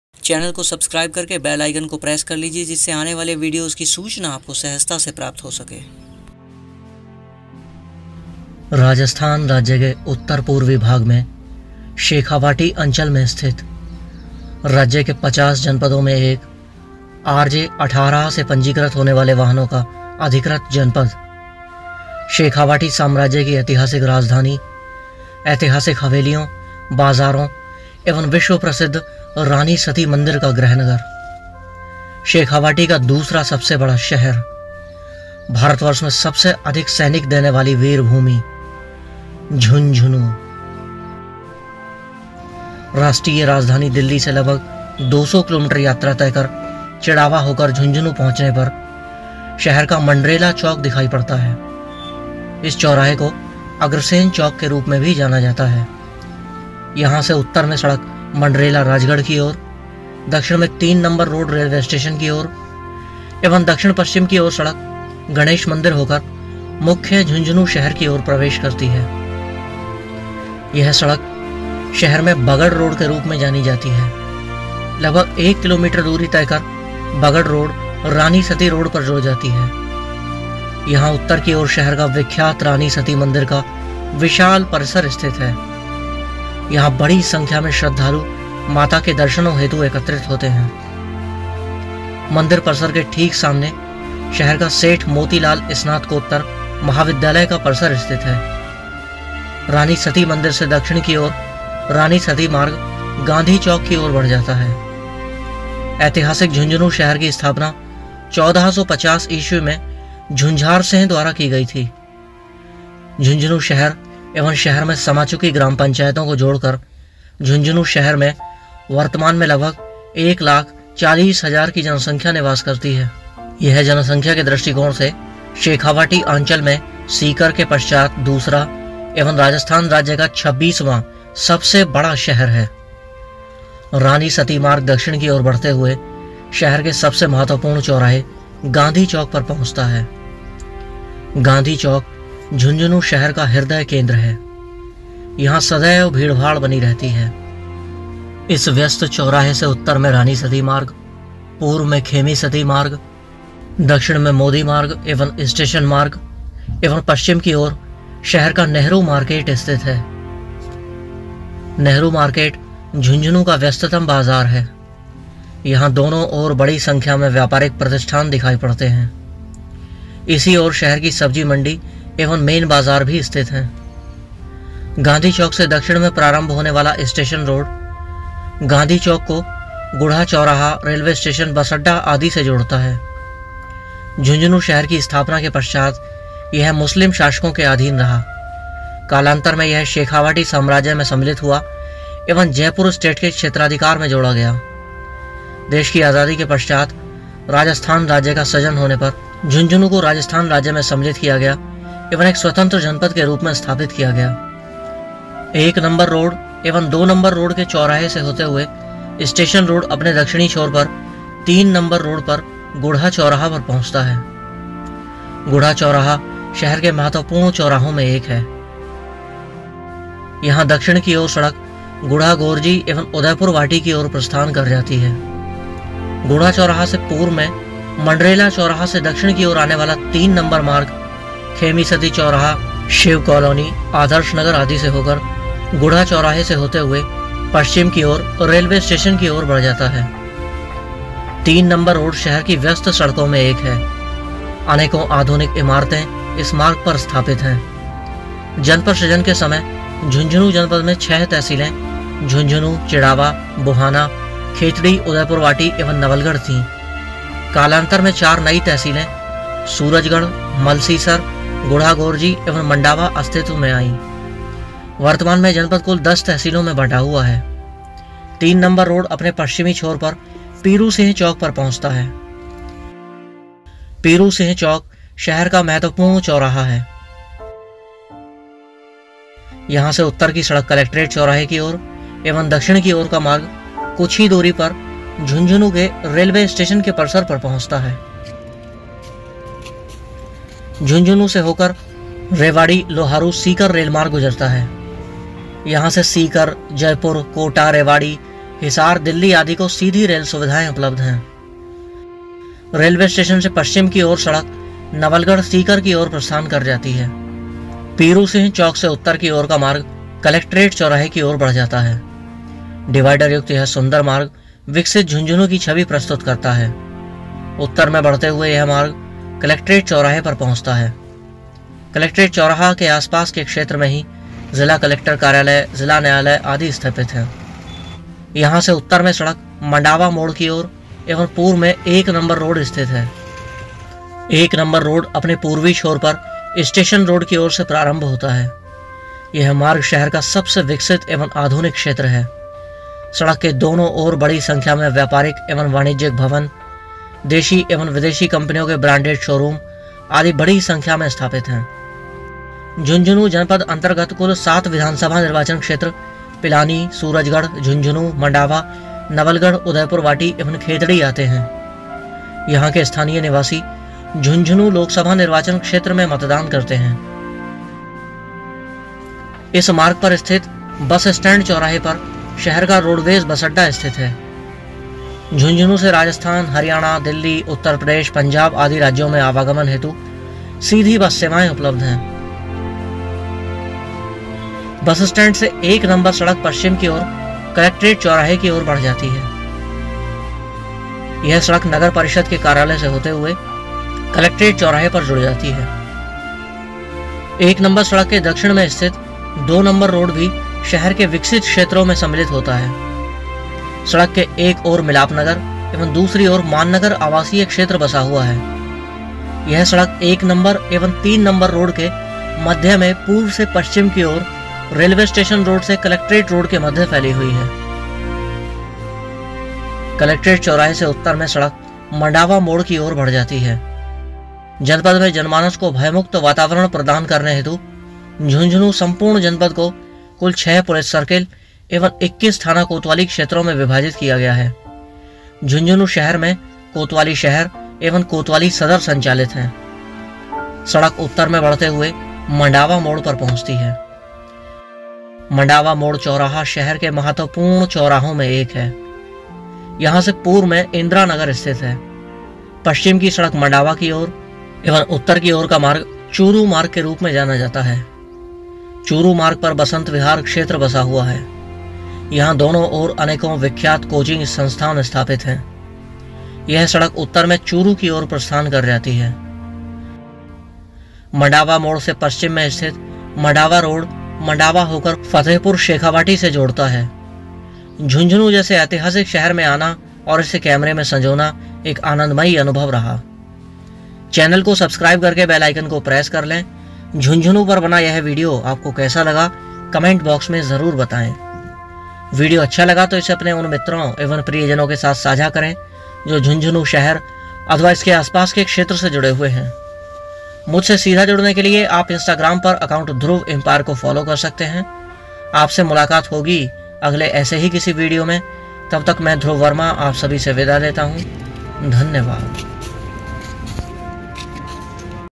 JHUNJHUNU CITY झुन्झुनू शहर Jhunjhunu Rajasthan Jhunjhunu Jila Jhunjhunu
इस वीडियो में आप देखेंगे राजस्थान राज्य के झुन्झुनू शहर का वर्णन